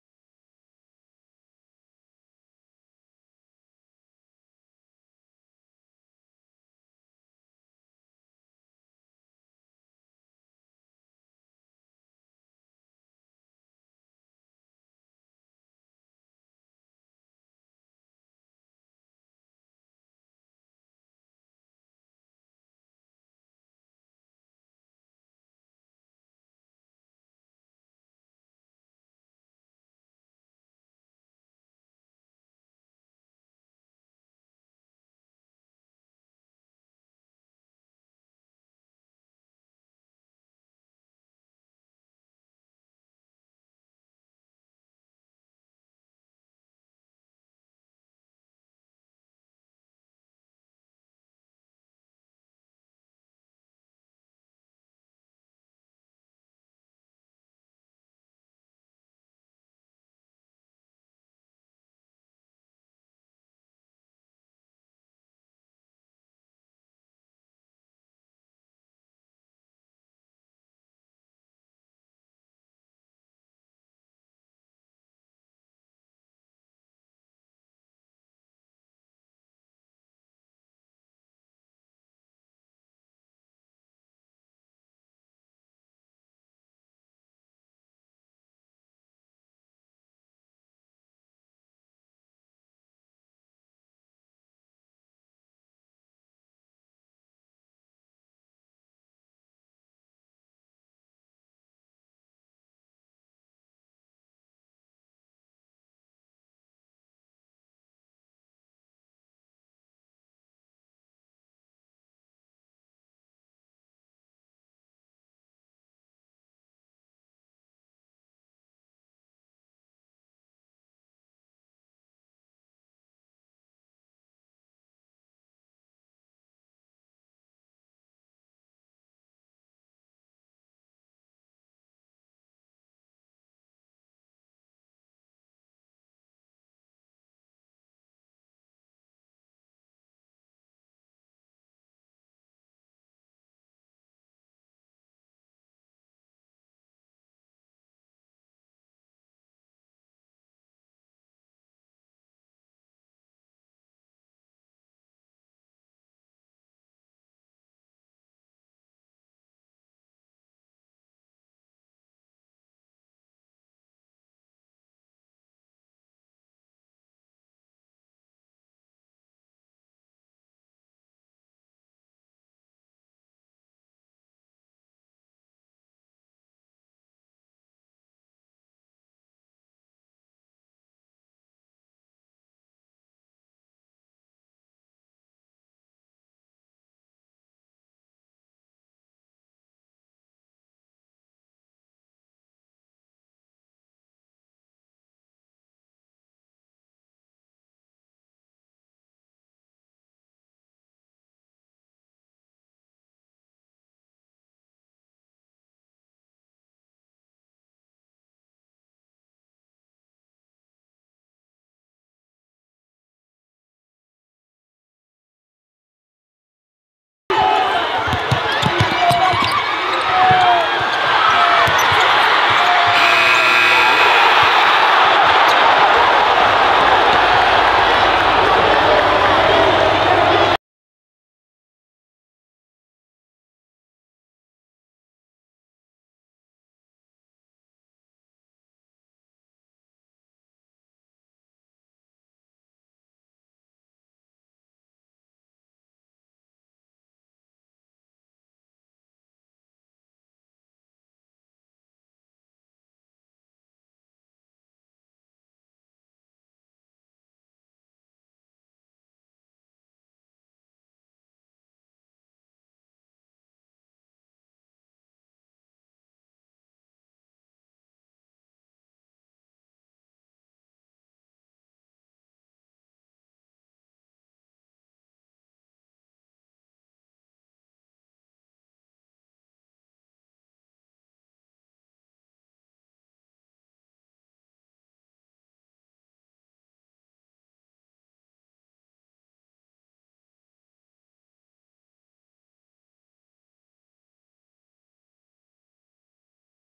Sahvir Wheeler Washington Huskies 19 PTS 14 AST 4 REB vs Seattle U | Pac 12 Player of the Week!
📺 New to Next Ones? Start Here!

🏀 Are you a Player or Organization?